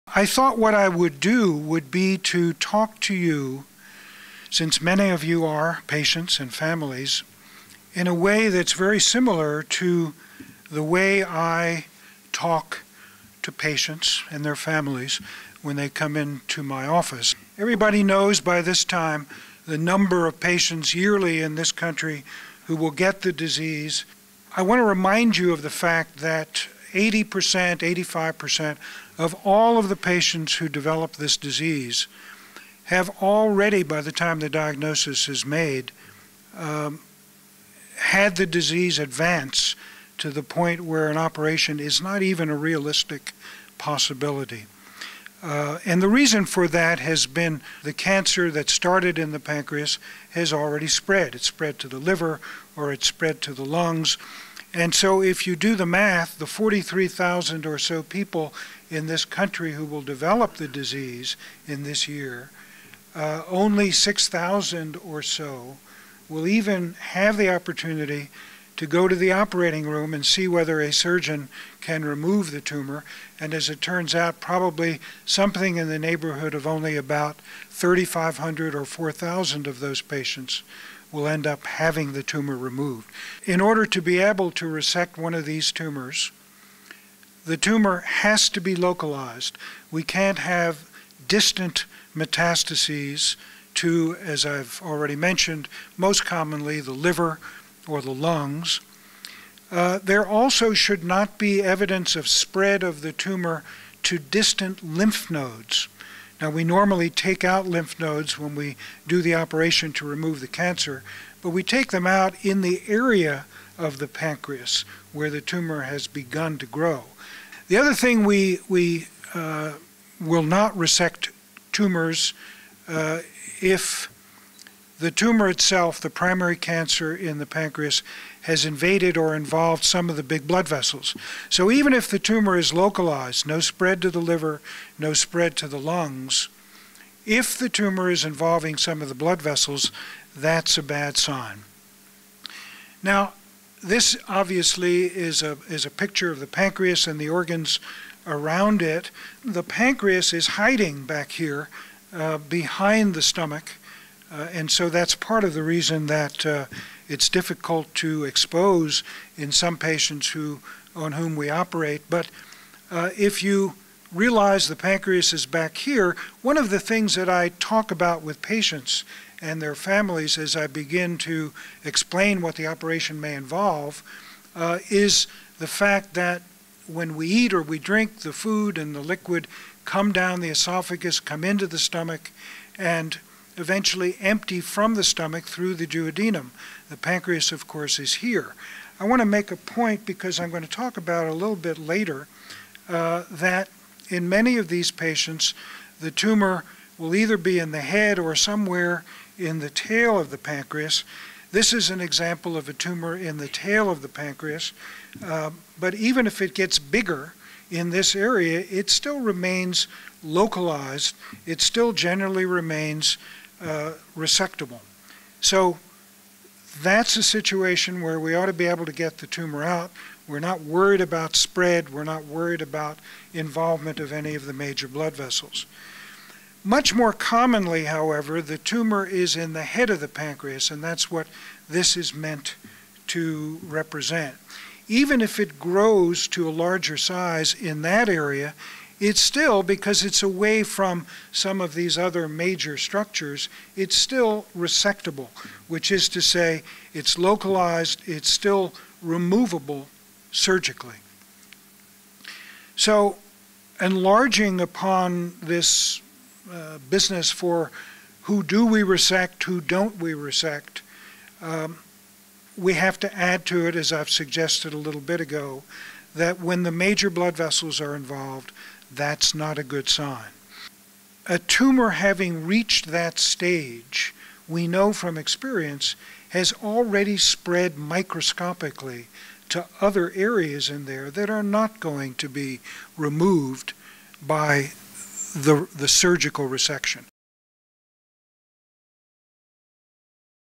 Dr Reber Speaks on Criteria for the Resection of Pancreatic Cancer
For surgeons, pancreatic cancer is the most challenging of the gastrointestinal malignancies. The tumor has spread in 80% of cases even before the diagnosis is made, so that surgical resection is an option in only about 20%.  The operation (Whipple procedure) is the most complex of general surgical procedures, and cures less than 20% of those who undergo it.  Nevertheless, some patients are cured by surgery (and additional medical therapy), and all patients who have ever been cured have had the tumor surgically removed.  Thus, surgical resection will remain a central feature in the management of this disease for the foreseeable future.